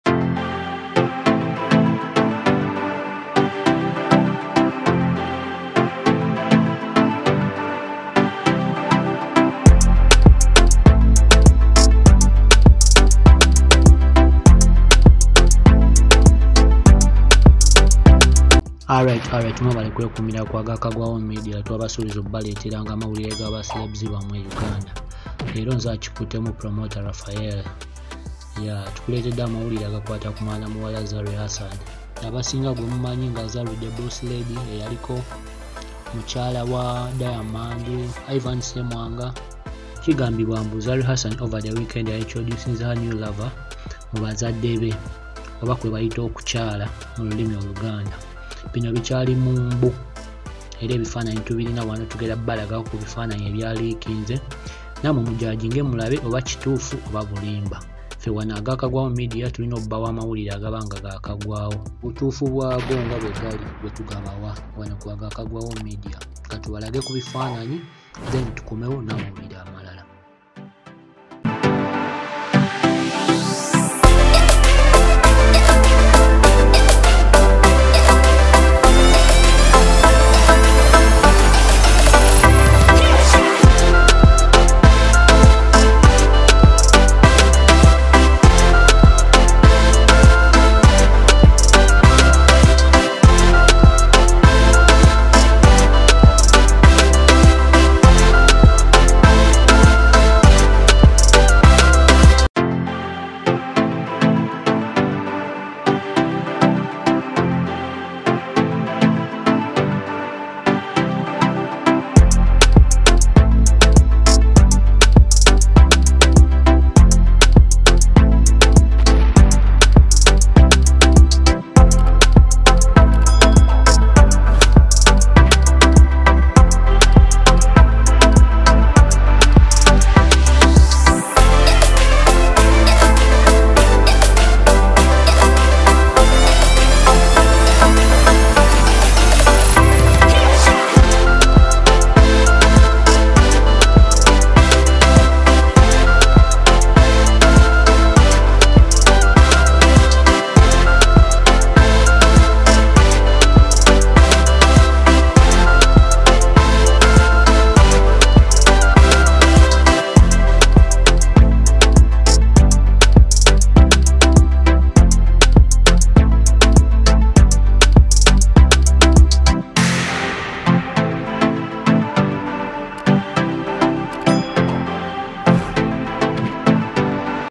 Zari Hassan Introduces New Lover To Parents In Secret Kukyala Ceremony! (Full Video)
Zari Hassan (The BossLady) Introduces New Lover To Parents In Secret Kukyala Ceremony! (Full Video)